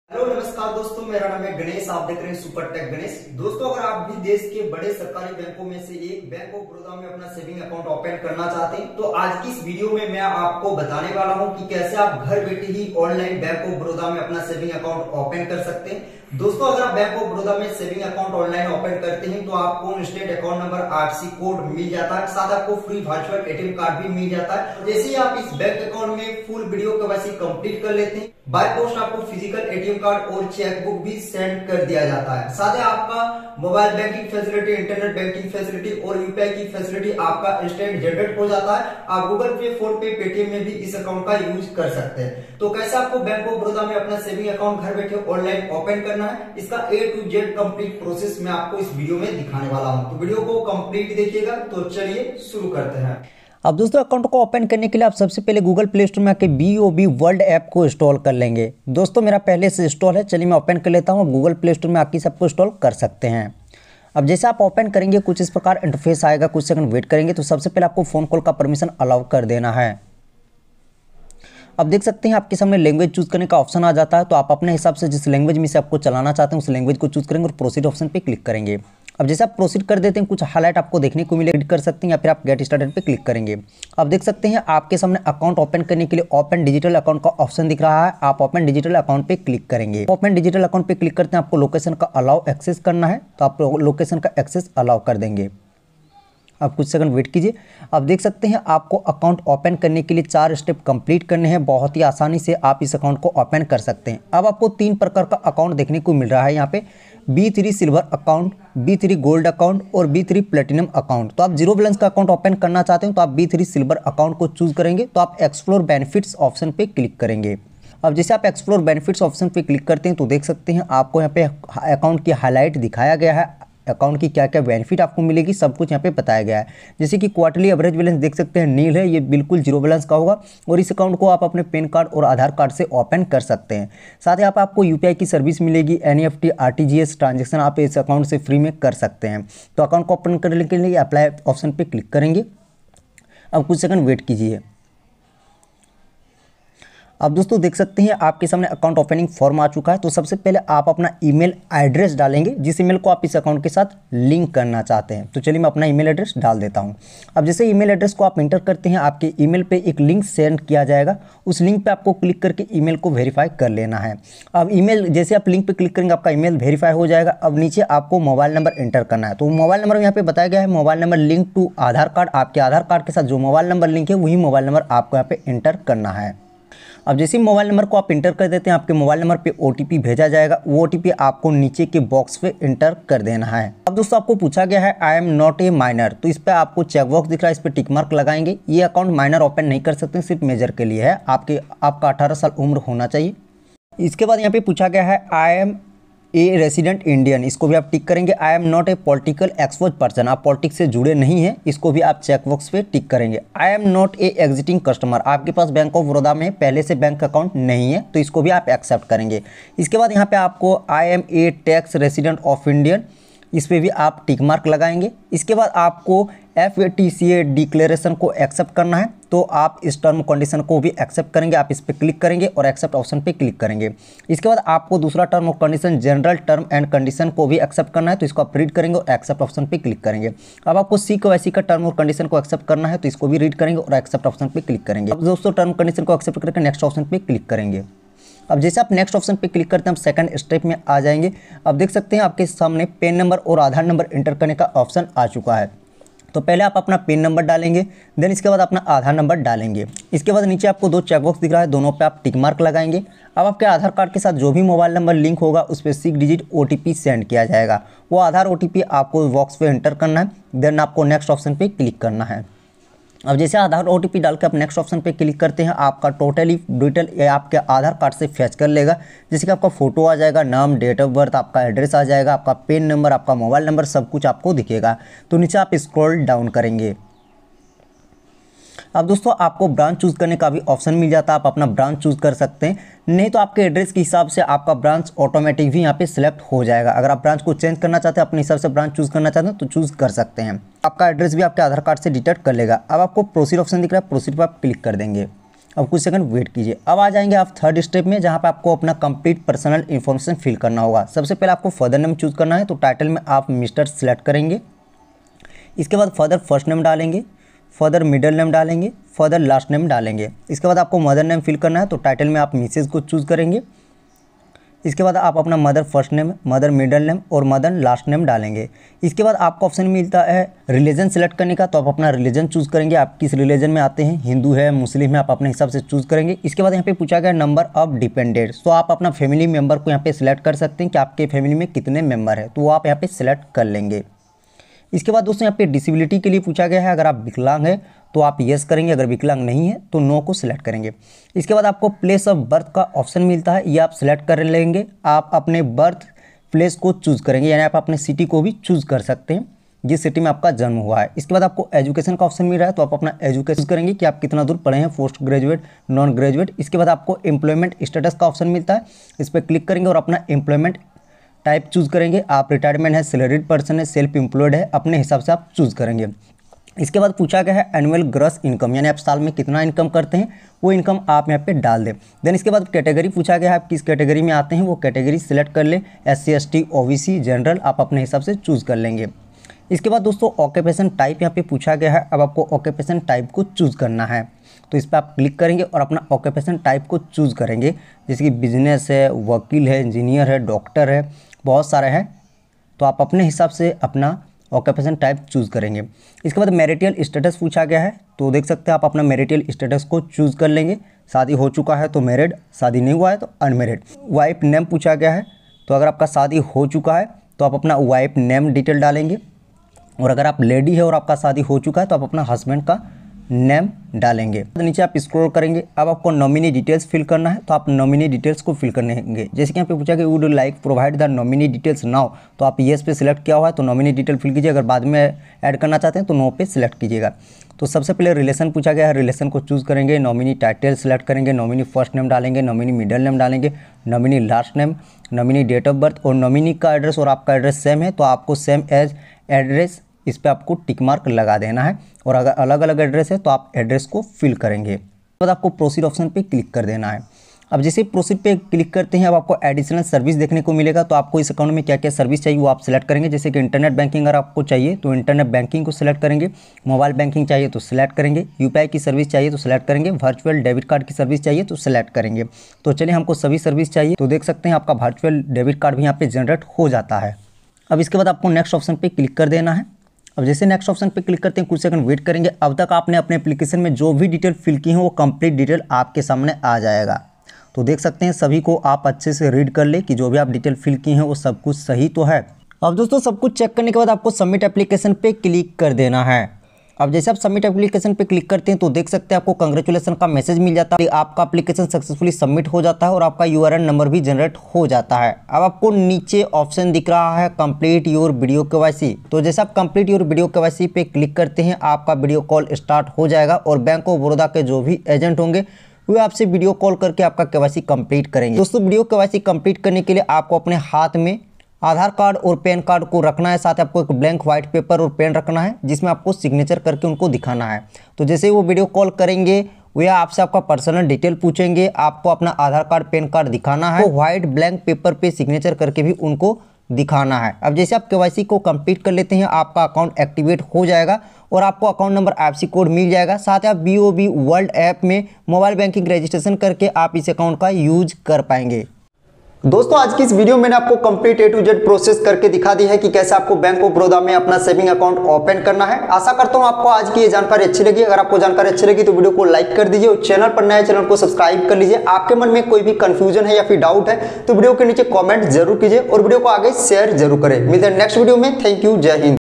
Bank of baroda online account opening | bank of baroda zero balance open 2023
About this video- bank of baroda account opening online 2023 complete process dikhaya gaya hai.